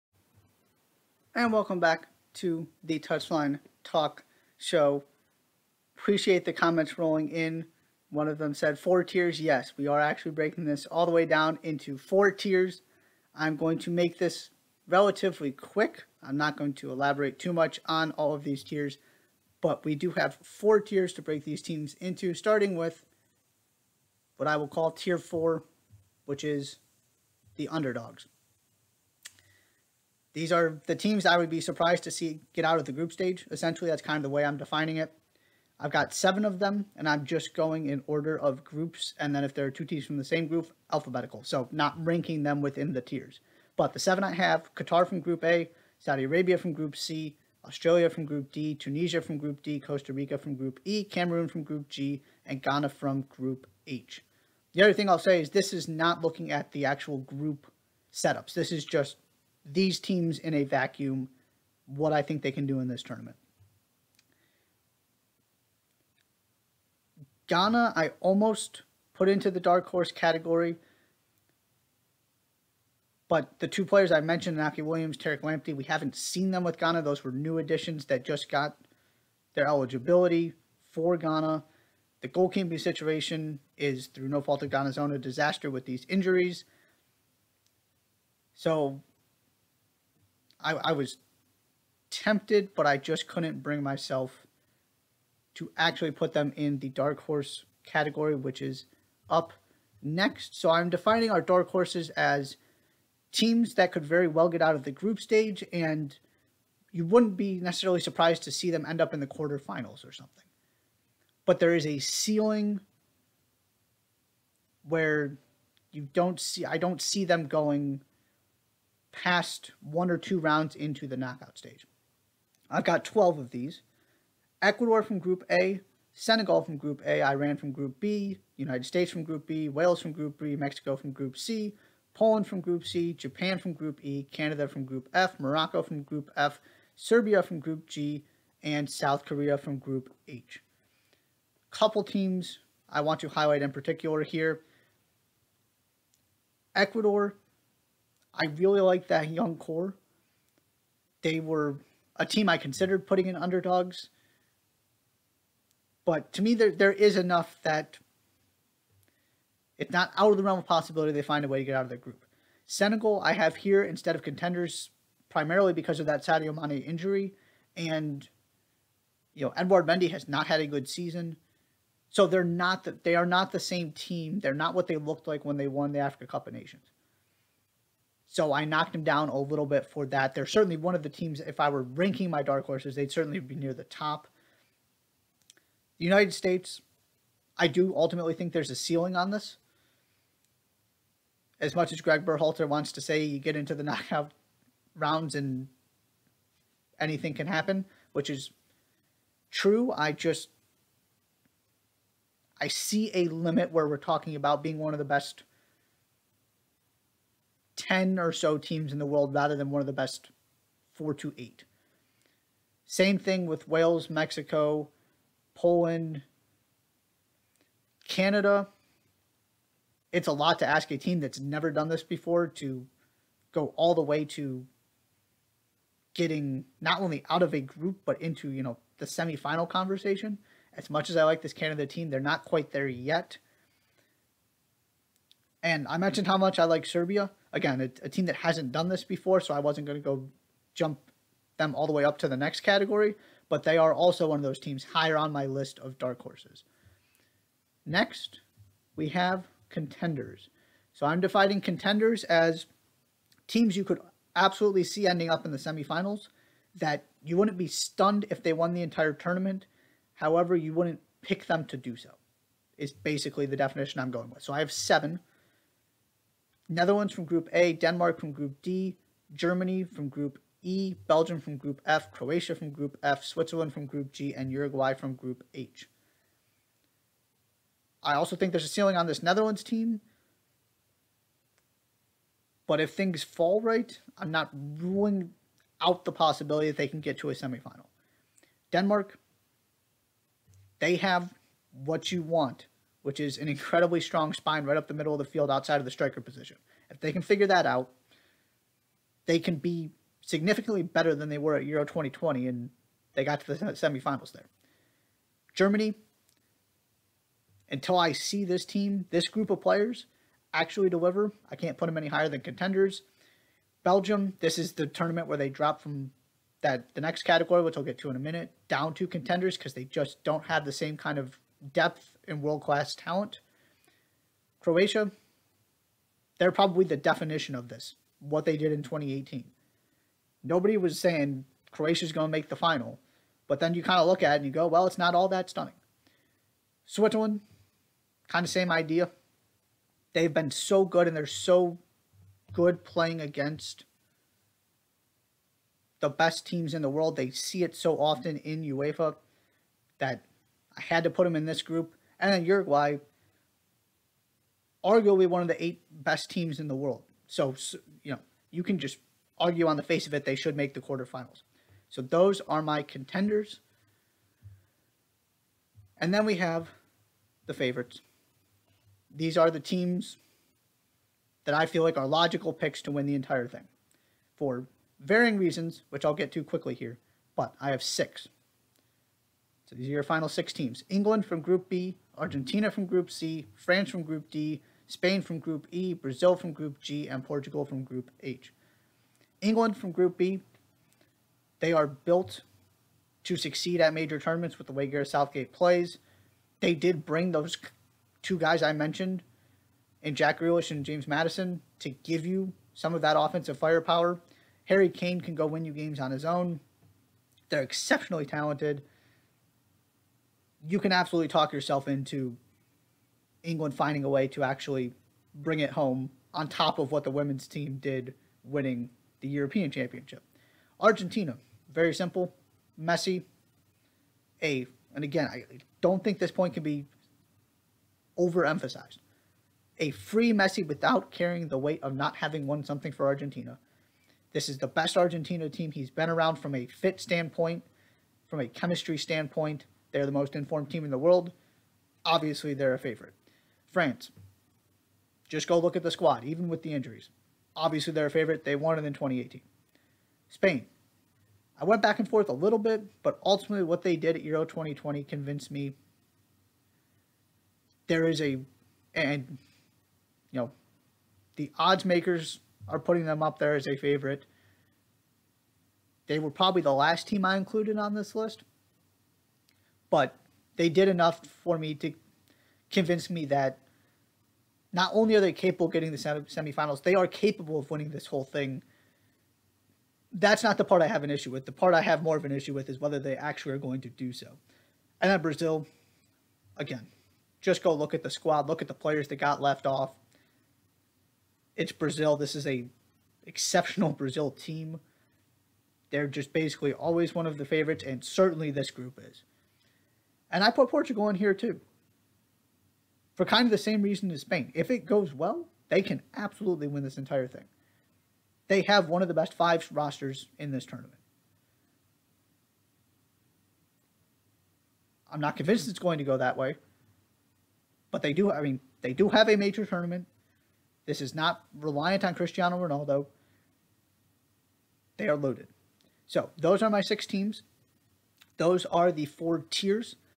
World Cup Tiers
Josh breaks the 32 World Cup teams into four tiers: underdogs, dark horses, contenders and favorites.